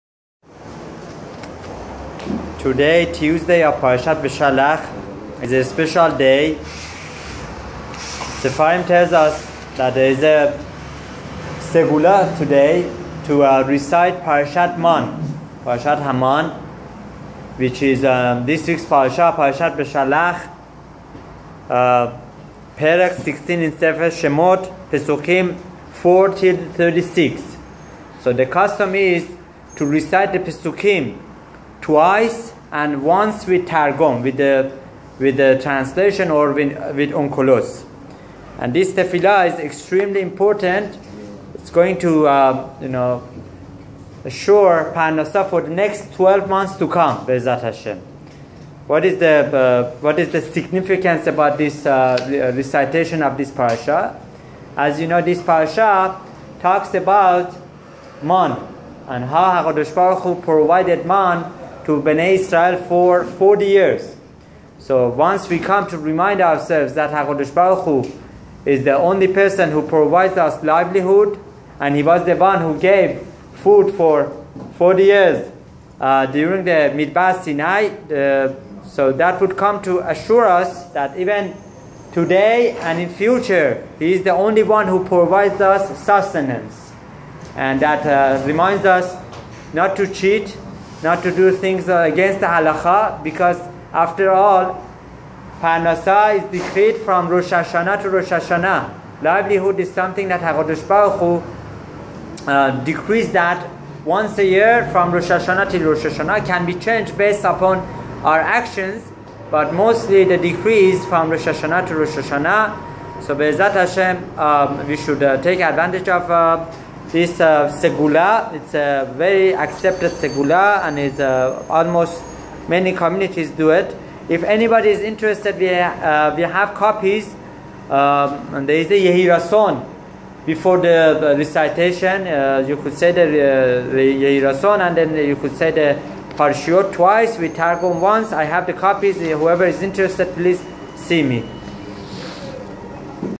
Parashat Manna פרשת המן Segula for Parnasah Livelihood.
Segula for livelihood :There is a accepted custom of reciting Parashat Hamon in Beshalach on Tuesday of the week of Parashat Bshalach. Here's the link to the text :